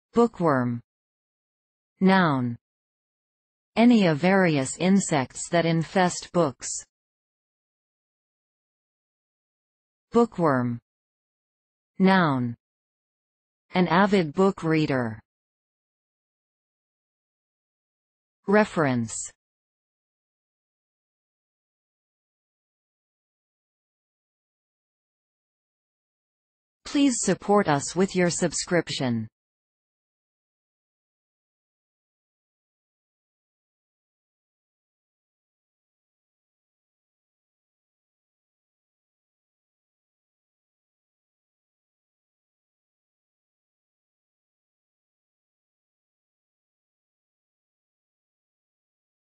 Bookworm | Definition of bookworm 📖 📖 📖
See here, the definitions of the word bookworm, as video and text.

bookworm (noun)
    Any of various insects that infest books.

bookworm (noun)
    An avid book reader.

Reference:
    bookworm